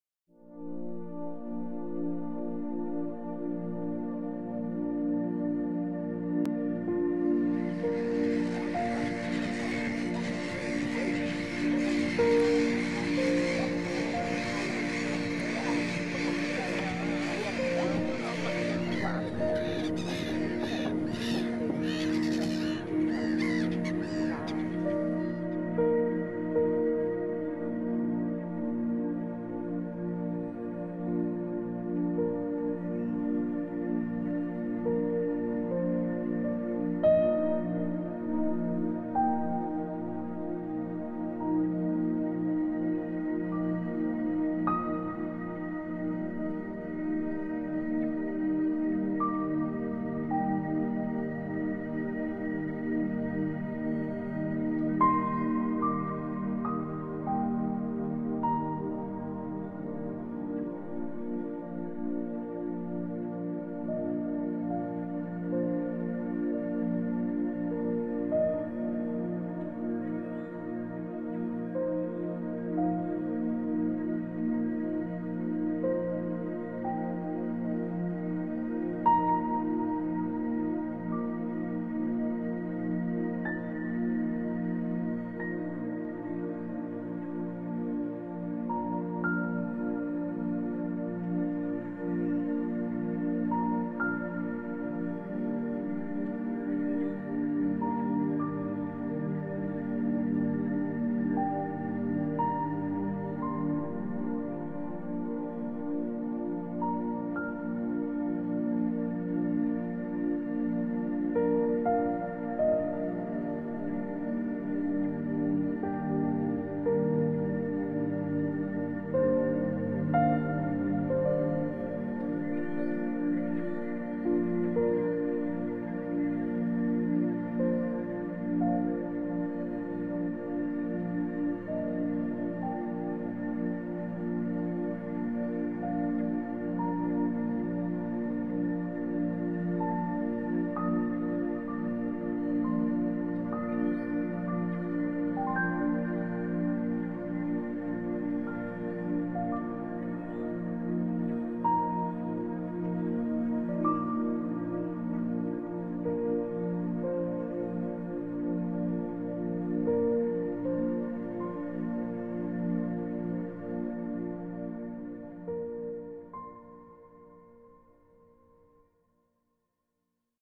nieuwkoopse plassen
The 'Nieuwkoopse plassen' is an oasis in the green hart of the Netherlands, full of birdlife. I shot this short film on a boattrip excursion discovering the area.